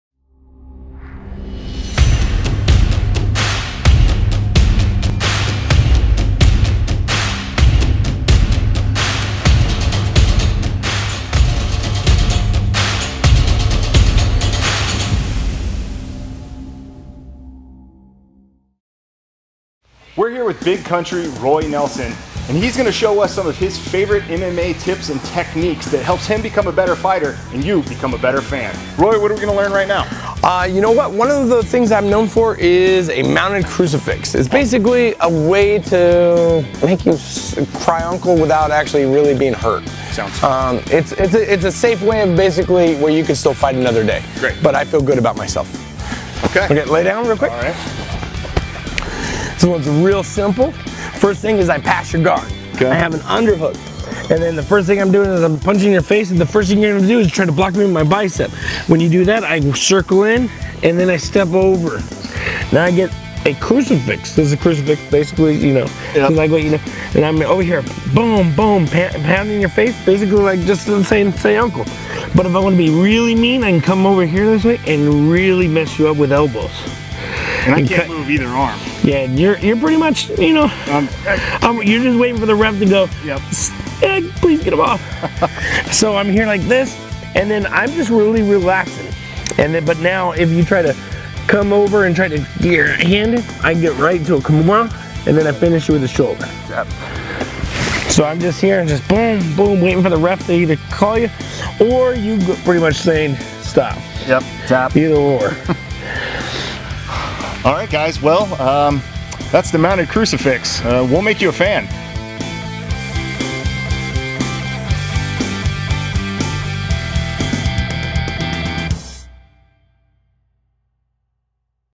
Roy Nelson demonstrates The Mounted Crucifix
Are you a fan of Mixed Martial Arts? We'll make you one. Boston's fighter Roy "Big Country" Nelson shows you some of his favorite MMA moves and techniques as he trains for his upcoming fight on September 23rd.